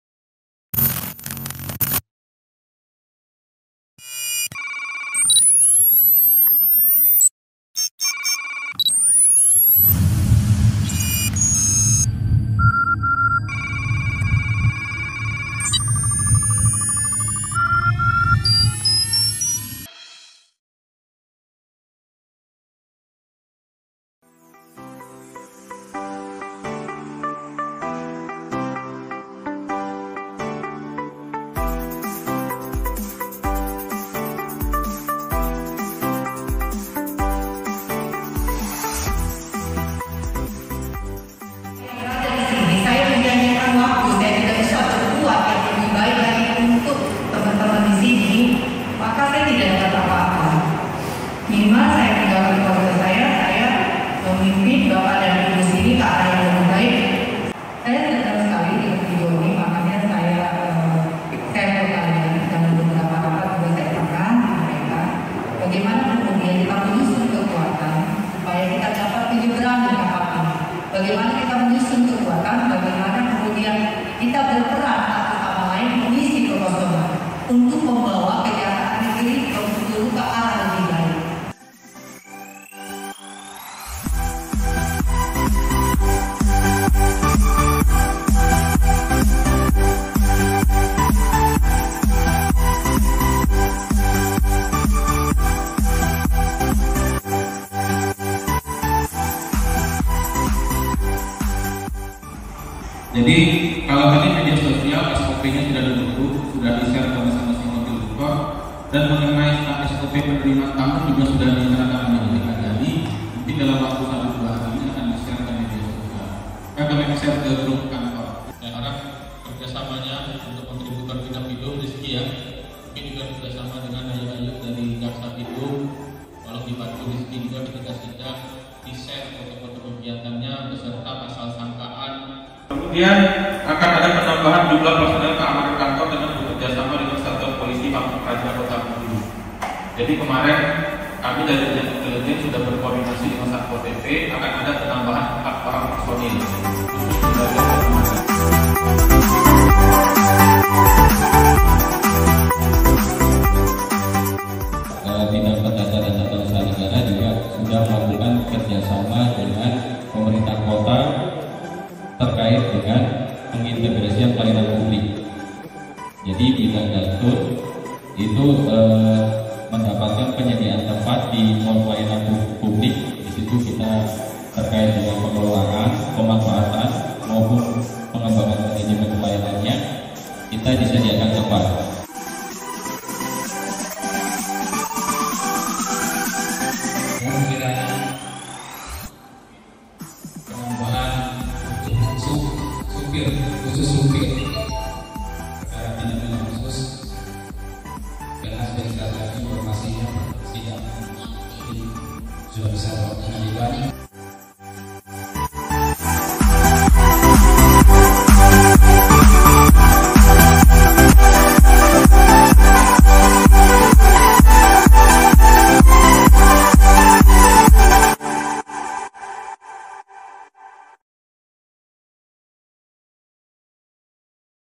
Rapat Pembahasan Kinerja Kejaksaan Negeri Bengkulu tahun 2022
Kejaksaan Negeri Bengkulu melaksanakan kegiatan pembahasan kinerja Kejaksaan Negeri Bengkulu untuk tahun 2022 yang diikuti oleh seluruh Pegawai dan Tenaga Honorer pada Kejaksaan Negeri Bengkulu bertempat di Aula Kejaksaan Negeri Bengkulu